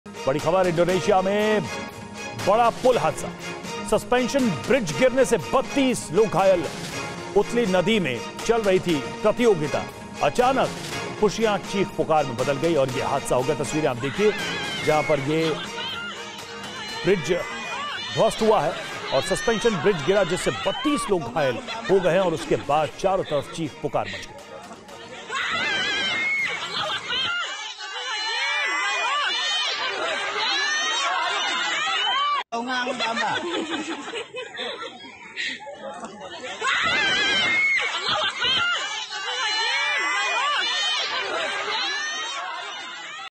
Bridge Accident In Indonesia: Indonesia में बड़ा पुल हादसा, Suspension Bridge गिरने से 32 लोग घायल
इंडोनेशिया में बड़ा पुल हादसा, सस्पेंशन ब्रिज गिरने से 32 लोग घायल, उथली नदी में चल रही थी प्रतियोगिता. अचानक खुशियां चीख-पुकार में बदली.
Big bridge accident in Indonesia, 32 people injured due to suspension bridge collapse, competition was going on in shallow river. Suddenly the happiness turned into screams.